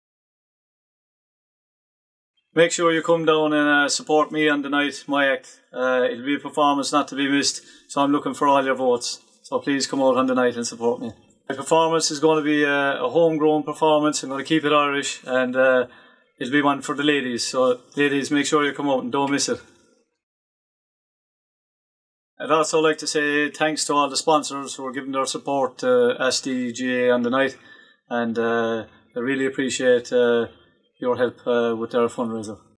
Eoin OMahony Video
Eoin O'Mahony - one of the acts for Asdee GAA's Stars in Their Eyes night.